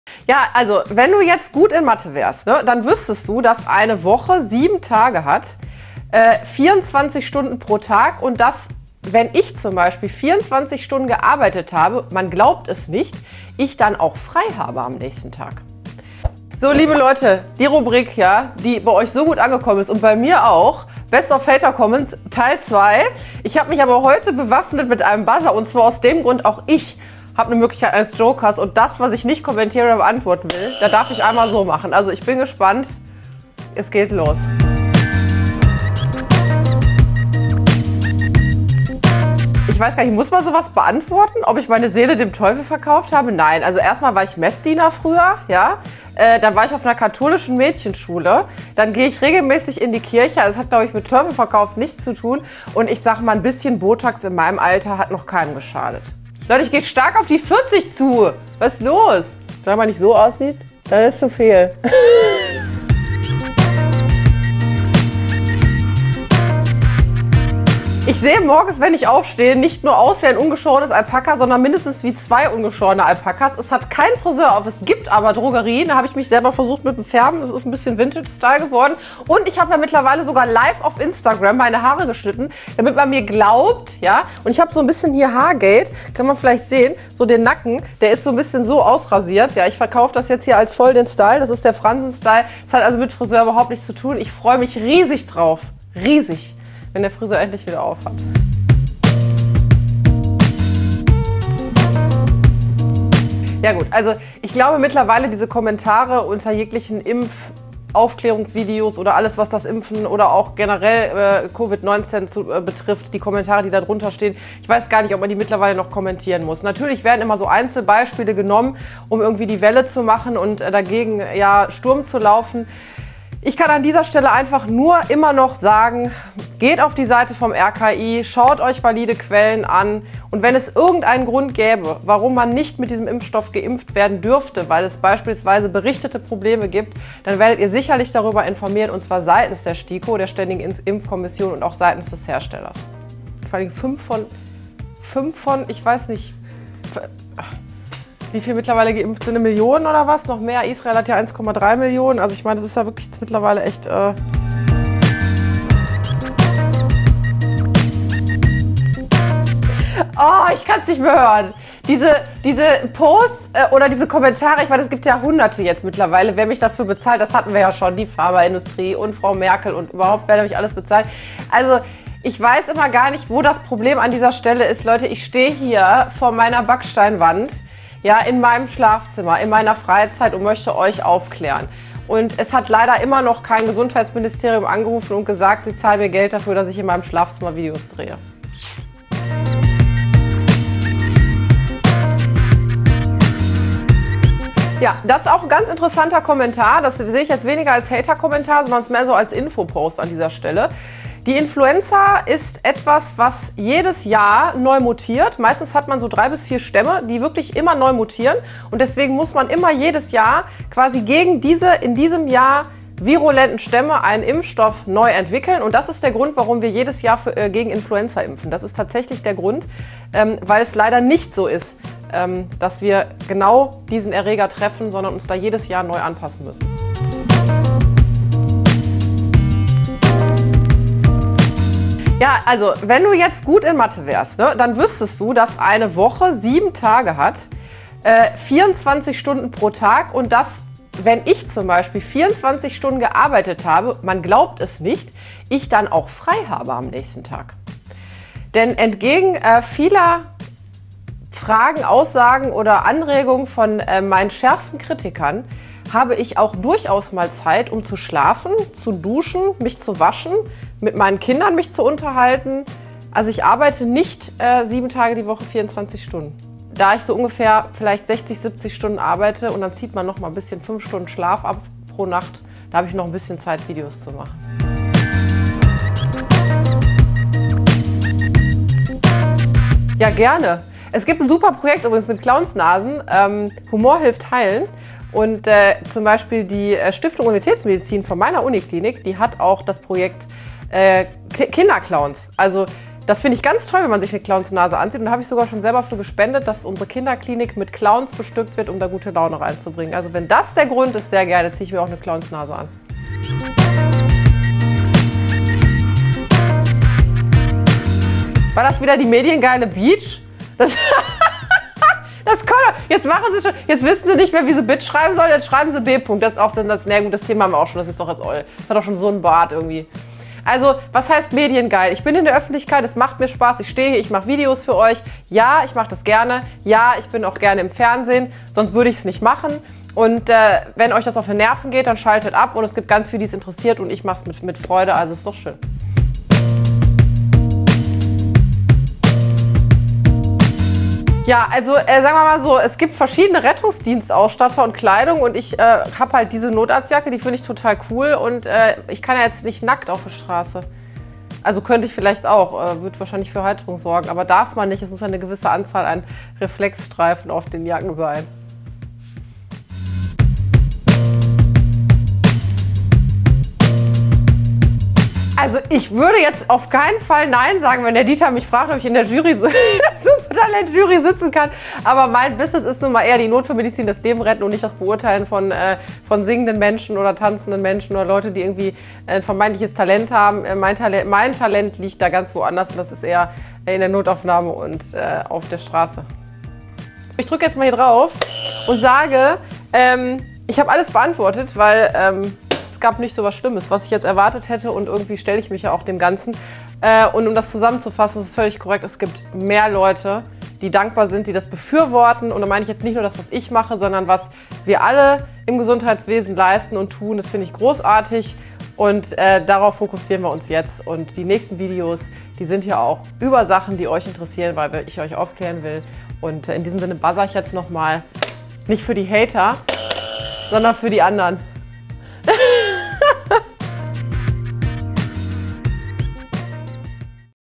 TALK I BEST OF HATER COMMENTS I TEIL 2
In diesem Talk antworte ich wieder auf die "besten" Hater Comments. Schaut wieder rein und ich freue mich natürlich, wenn ihr eure Favoriten und auch neue Themenvorschläge in die Kommentare schreibt.

Bleibt gesund!